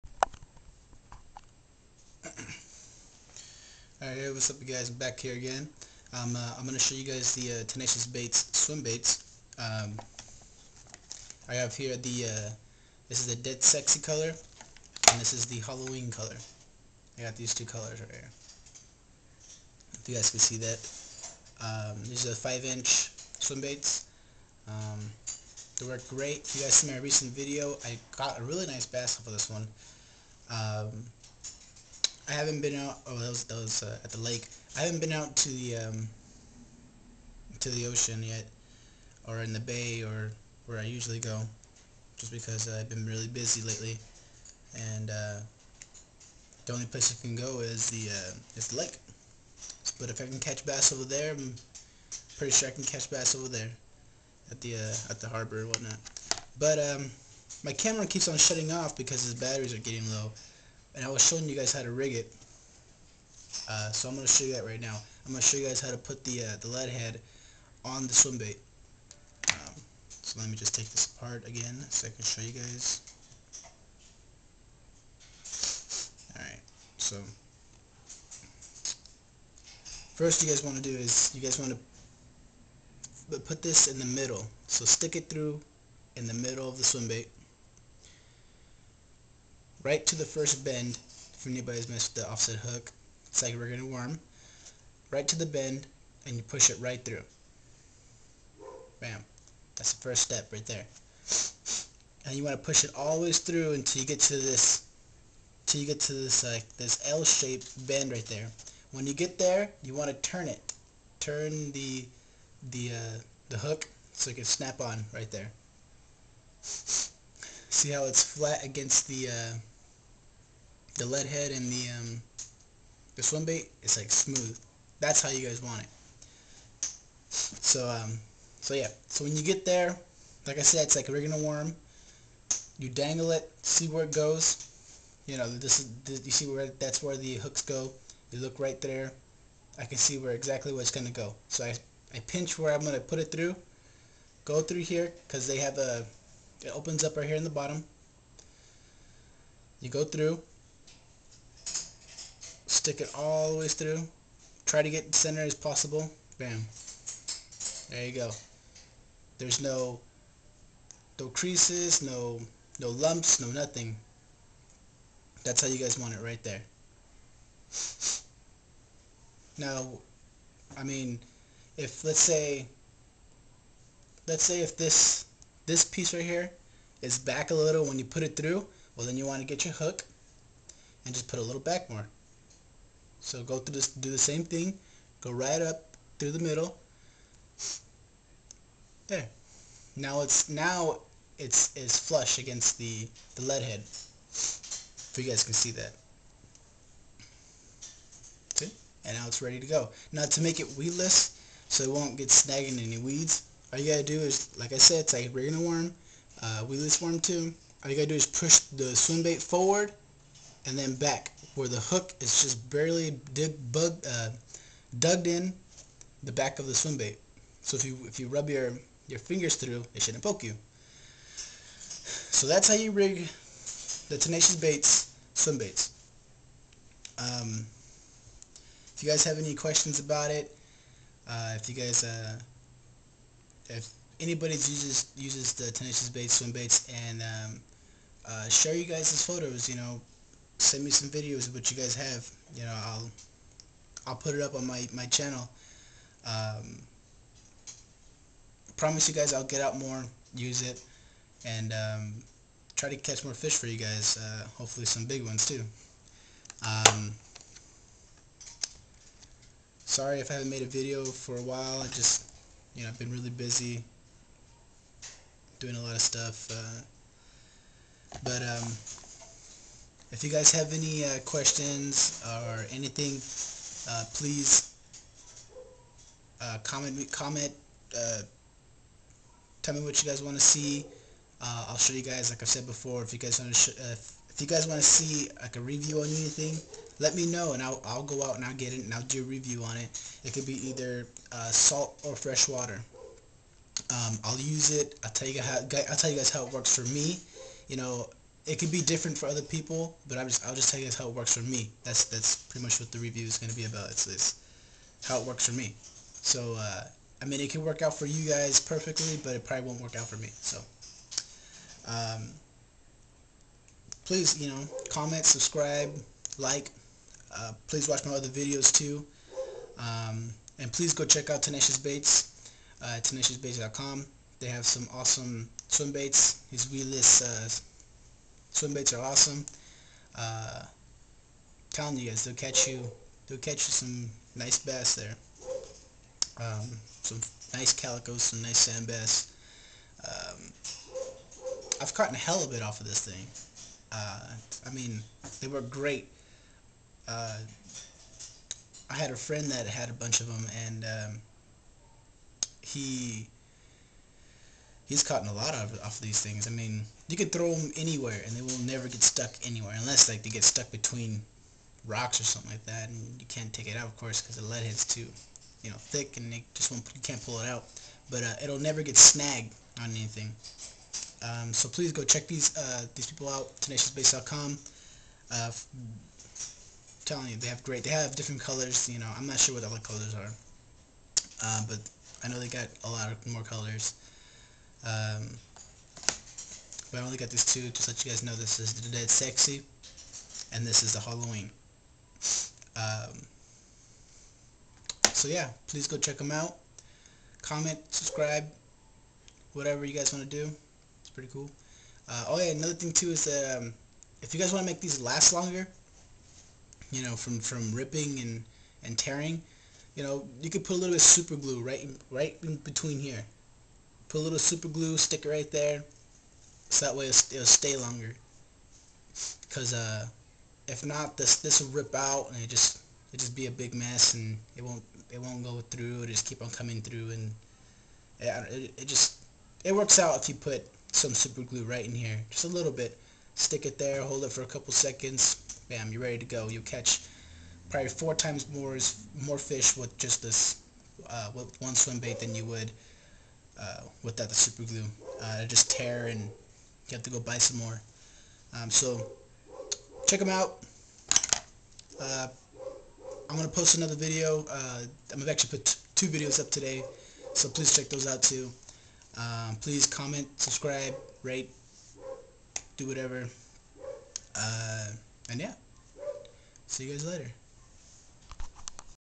FISHING TATICS TV # 10 TENACIOUS BAITS #2
today im going to show you how to rig the tenacious bait swimbait on the led head. tenaciousbaits.com